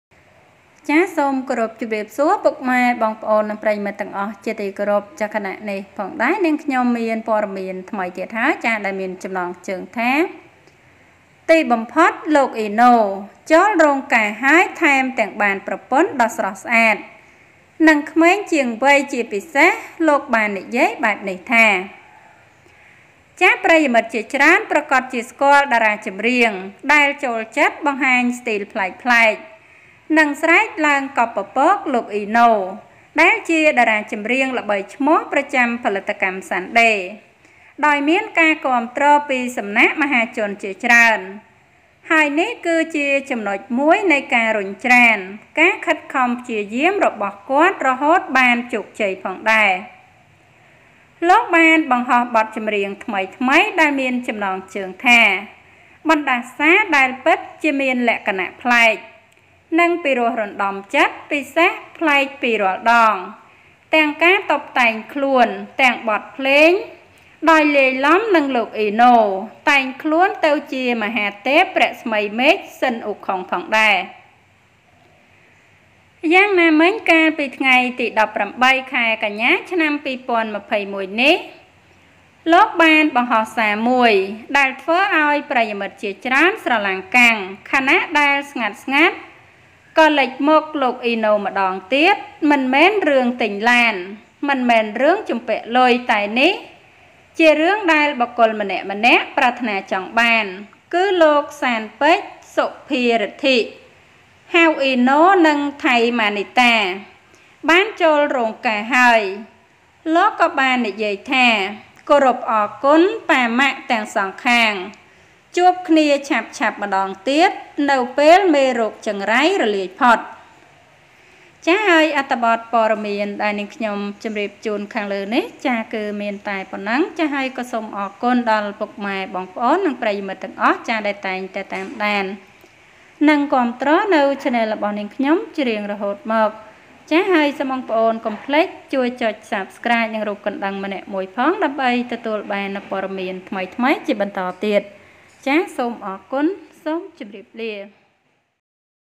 Eno finally got married and even got a beautiful and younger wife, especially he said:
ទីបំផុត លោក អ៊ីណូ ចូលរោងការហើយ ថែមទាំងបានប្រពន្ធដ៏ស្រស់ស្អាត និងក្មេងជាងវ័យ ជាពិសេសលោកបាននិយាយបែបនេះថា…